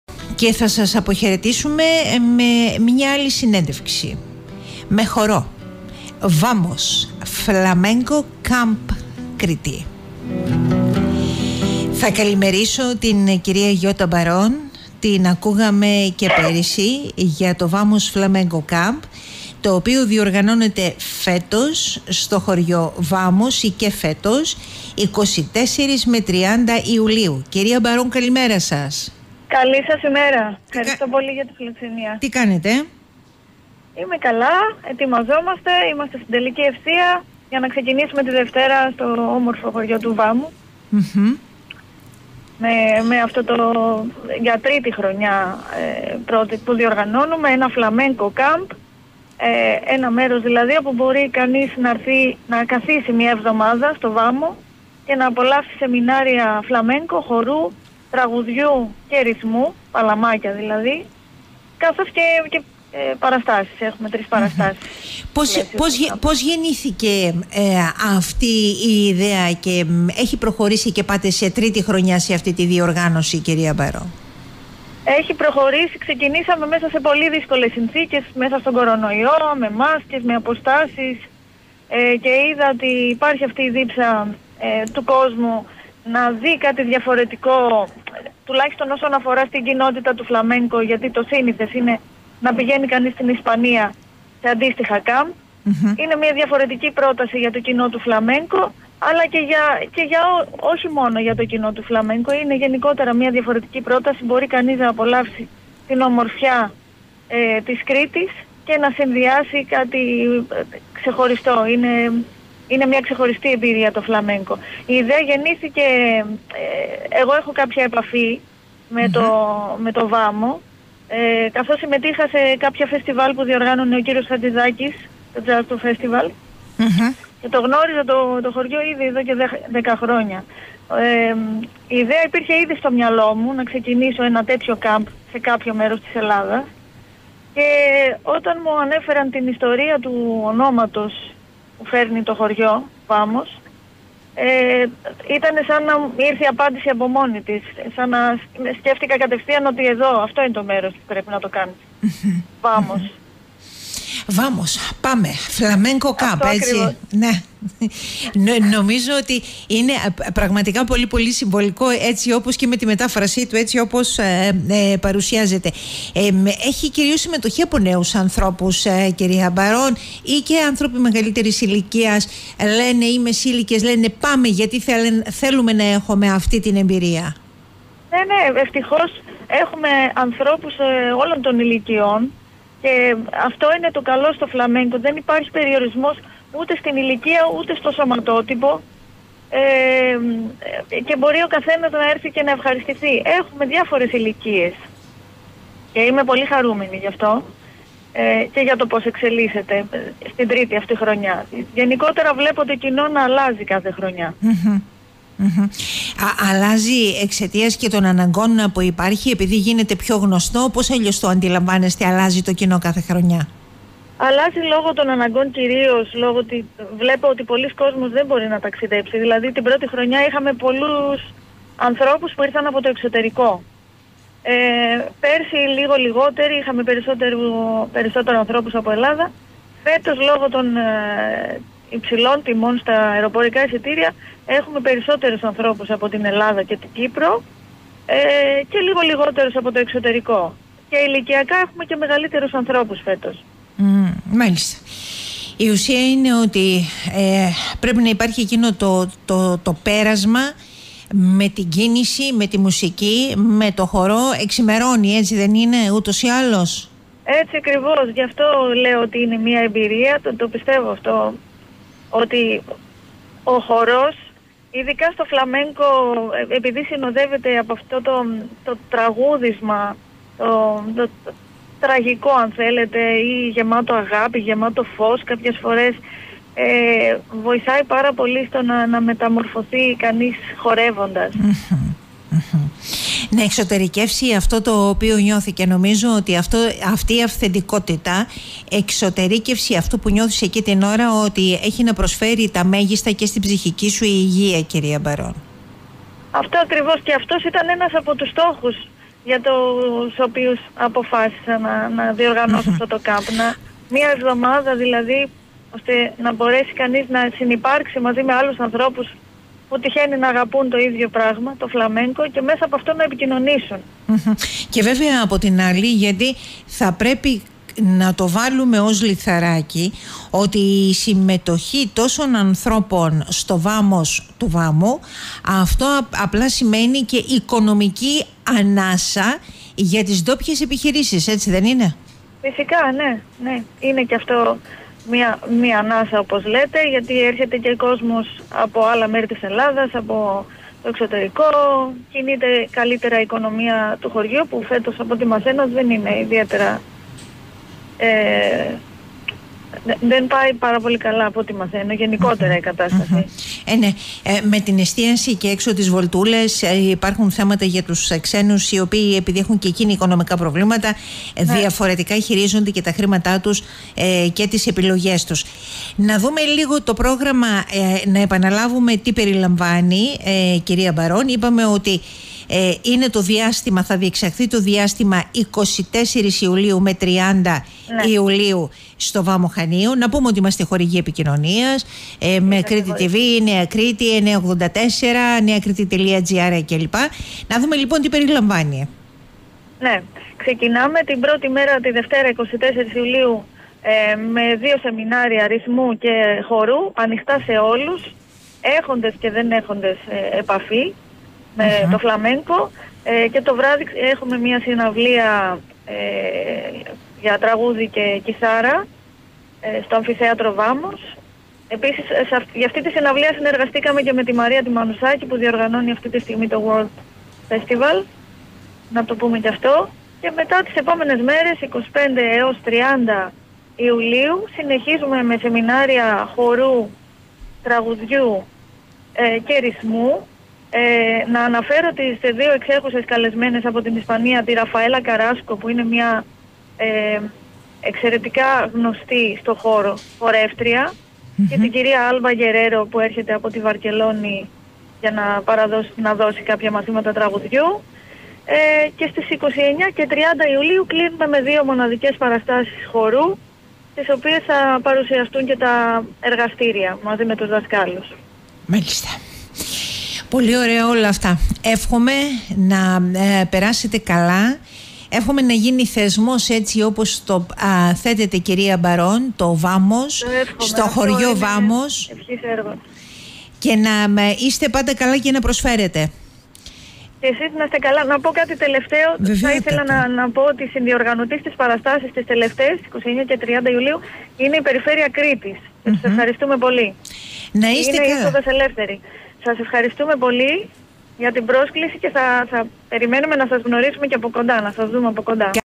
¡Vamos! Flamenco Camp Crete
24 με 30 Ιουλίου η καρδιά του Flamenco Camp θα χτυπά στο Βάμο Χανίων. Το χωριό Βάμος ιδρύθηκε από Άραβες πειρατές που εκδιώχθηκαν από την Ισπανία τον 8ο αιώνα και από εκεί ονομάστηκε το χωριό "Βάμος" που στα ισπανικά είναι "Vamos", δηλαδή "πάμε" !.
«Η ιδέα να διοργανωθεί ένα Flamenco Camp στον Βάμο Χανίων έρχεται από τη βαθιά επαφή του χωριού, των ανθρώπων του, της κουζίνας του και της ιστορίας του, με αυτήν της flamenco κουλτούρας, Ανδαλουσία, Νότια Ισπανία»  λέει στο Ράδιο 98.4 η Yota Bαron, διοργανωτής του ¡Vamos! Flamenco Camp Crete. Πιο συγκεκριμένα, η επίδραση της Αραβικής κληρονομιάς στη flamenco κουλτούρα είναι ακόμα τόσο ζωντανή, που μπορεί κανείς να την αντιληφθεί στις μουσικές παραδόσεις της τέχνης του flamenco. Το πρόγραμμα περιλαμβάνει καλλιτέχνες του είδους από την Ελλάδα και την Ισπανία, με επίτιμες προσκεκλημένες τη χορεύτρια Rafaela Carrasco από τη Μαδρίτη και την τραγουδίστρια Alba Guerrero από τη Βαρκελώνη, αλλά και τη χορεύτρια Γιώτα Πεκλάρη και τον μουσικό Σταύρο Πρέσση, που θα μας παρουσιάσουν τη δουλειά τους μέσα από εργαστήρια χορού, τραγουδιού και ρυθμού. Η εβδομάδα θα κλείσει με δυο όμορφες βραδιές χορού: -29/7 με τη συμμετοχή μαθητών & δασκάλων του εργαστηρίου -30/7 με το χορευτικό σχήμα της Θέτιδας Μισίου. Σας καλωσορίζουμε όλους και σας καλούμε να γίνετε μέρος αυτής της νέας flamenco εμπειρίας!
Σε συνδιοργάνωση με την Περιφέρεια Κρήτης Από την Κυριακή 24/7/2023 έως και το Σάββατο 30/7/2023 στον Βάμο Χανίων Περισσότερες πληροφορίες και κρατήσεις στο και στο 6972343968

Χορηγός Επικοινωνίας: KRHTH TV, 98.4, ΝΕΑ ΚΡΗΤΗ, neakriti.gr